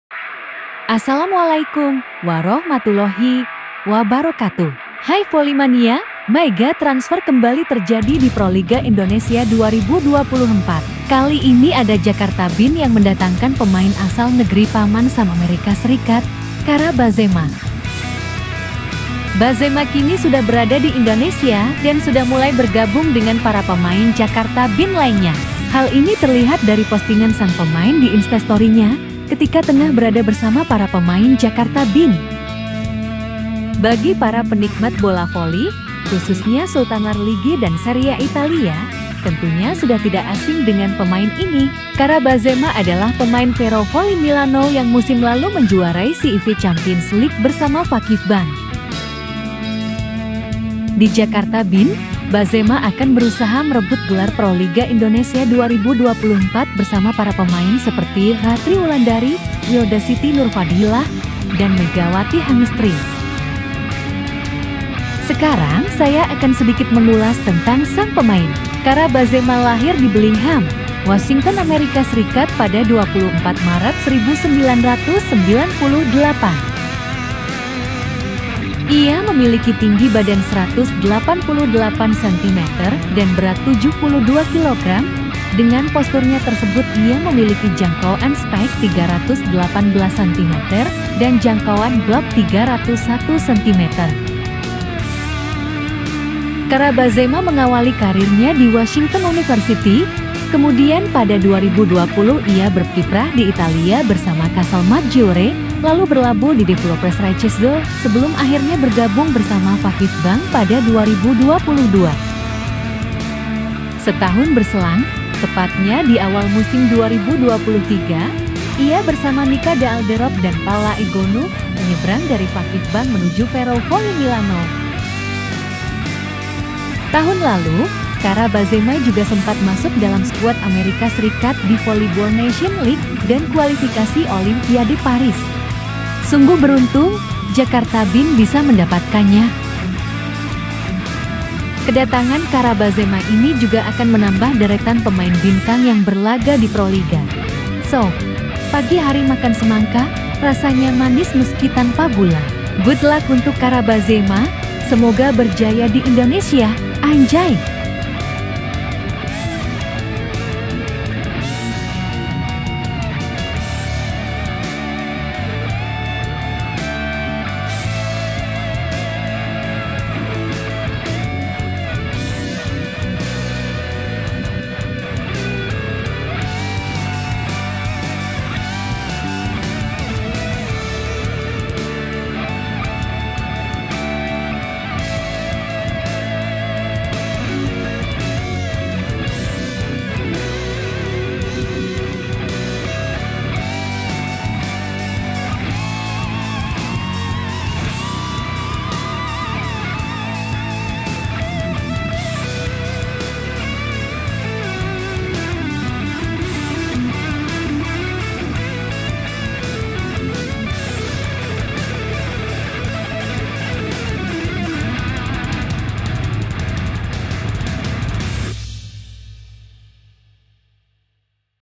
WOW, KARA BAJEMA BERGABUNG DENGAN JAKARTA BIN ‼️
Mega transfer kembali terjadi di proliga Indonesia, kali ini ada pemain asal Amerika Serikat yang bergabung dengan Jakarta BIN

bila ada kritik dan saran silahkan tinggalkan di kolom komentar atau bisa langsung DM ke instagram @gasukavolly